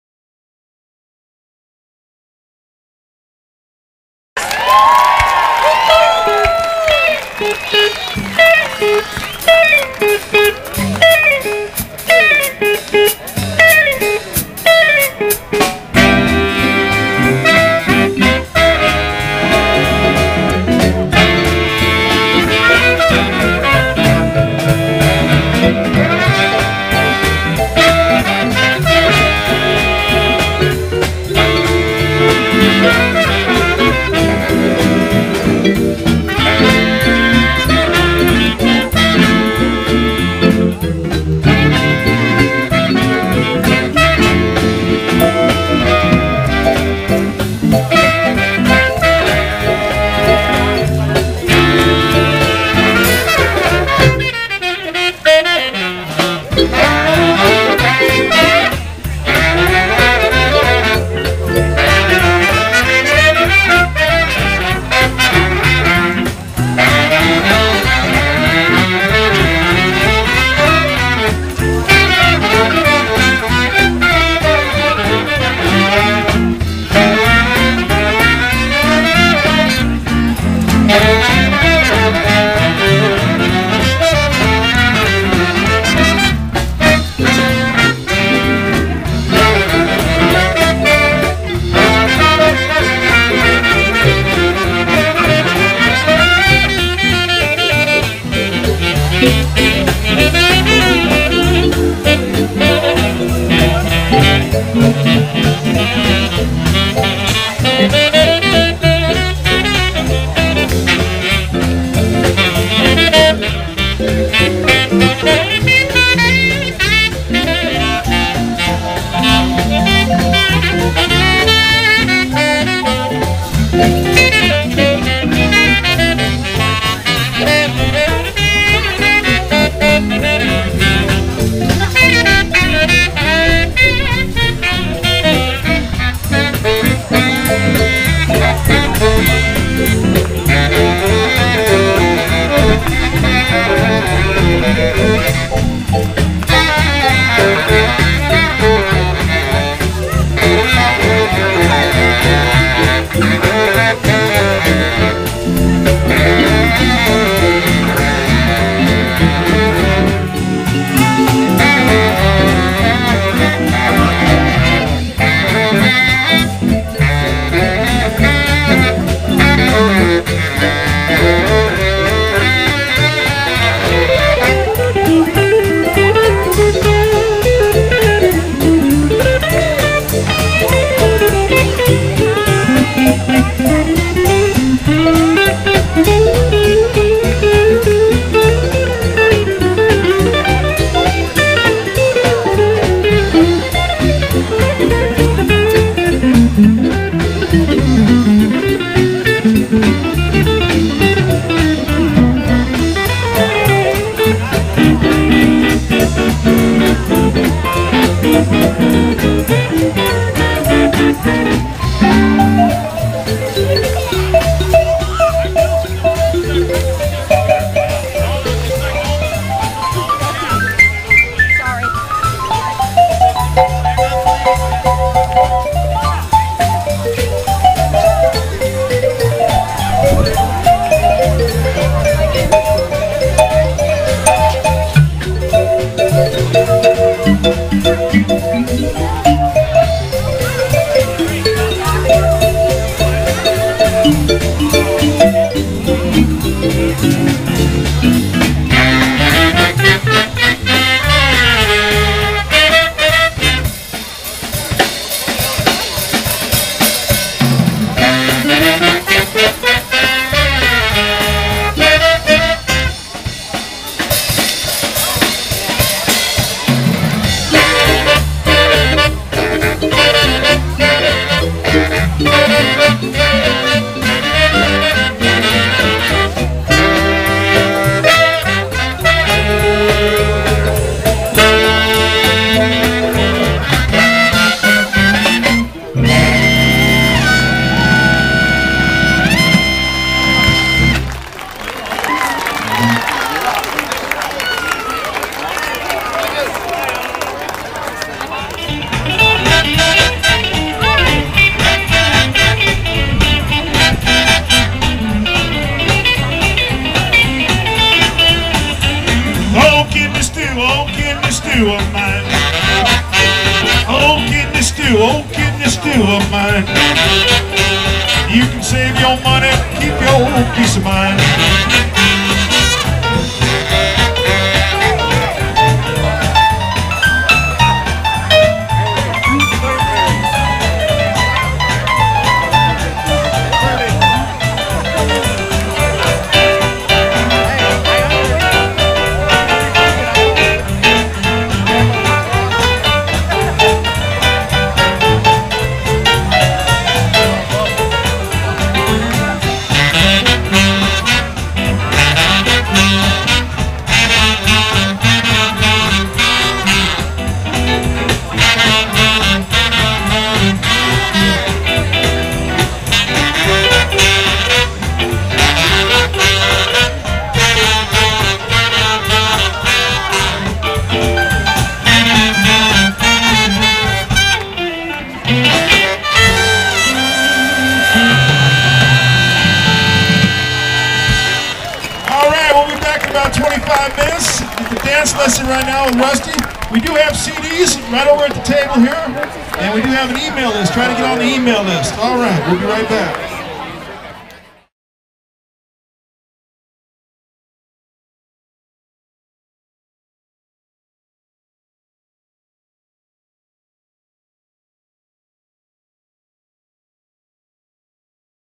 Flat Top Tom Concert at Polliwog Park in Manhattan Beach, CA on August 23, 2009
Swing dancers invaded Polliwog Park on Sunday for the Flat Top Tom concert (Big Band Swing & Blues).  The dancing never stopped in front of the stage and continued during the break when local dance instructor Rusty Frank taught a swing dance class.   Lots of Lindy Hop and East Coast Swing music with a splash of West Coast Swing.

Great music & Great dancing. Enjoy...!!!